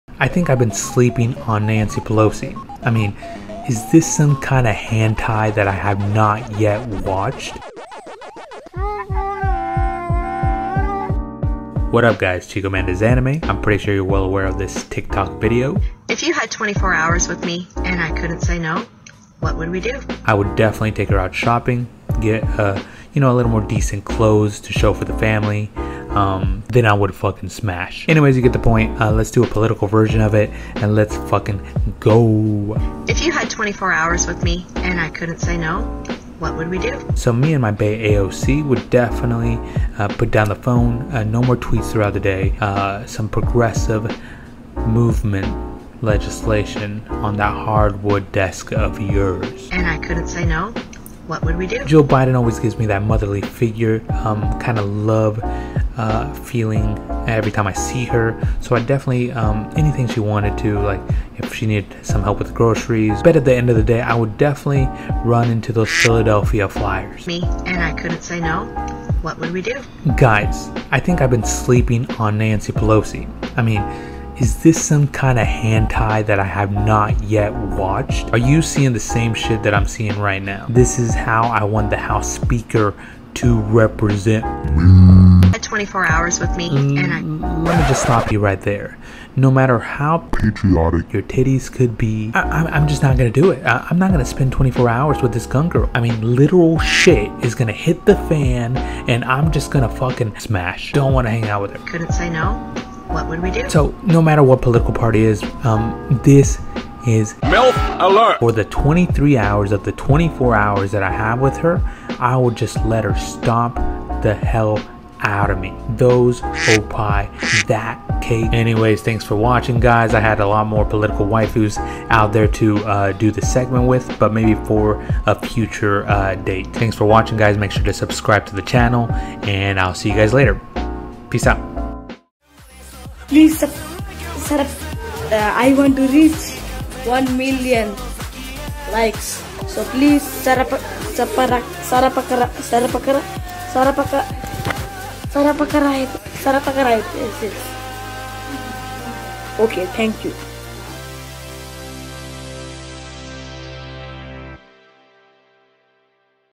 If you had 24 hours with me and i couldn't say no what would we do ? Political Edition
if you had 24 hours with your favorite politician and she couldn't say no what would you do to her?